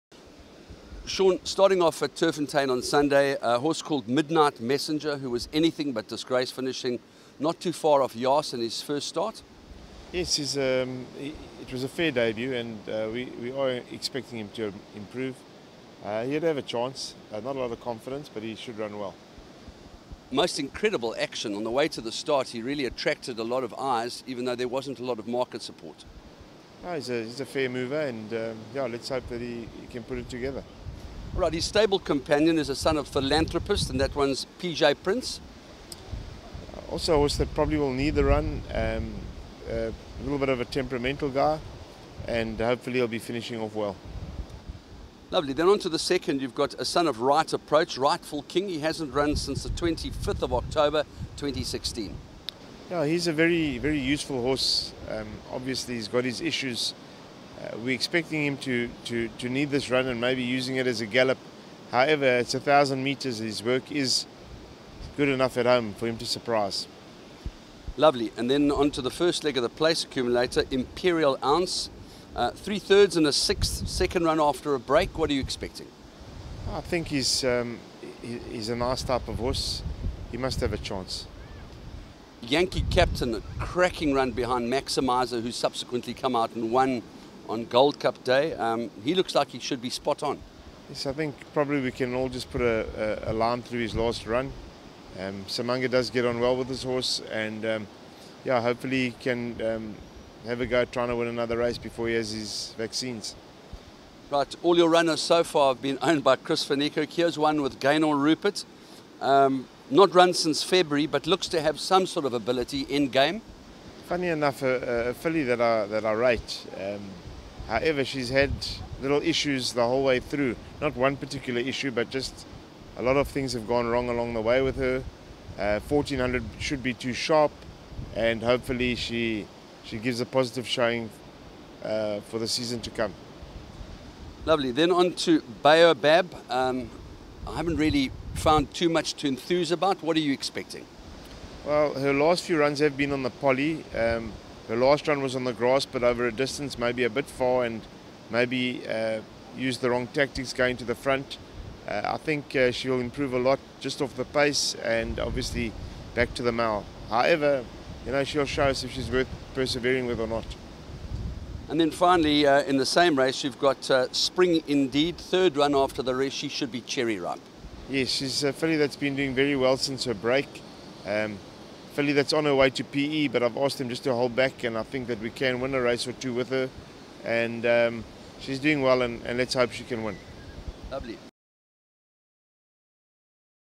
USO SUNDAY AUGUST 6TH TURF,SEAN TARRY 480P
MULTIPLE CHAMPION TRAINER SEAN TARRY EXPRESSES HIS VIEWS ON HIS RUNNERS FOR SUNDAY AT TURFFONTEIN.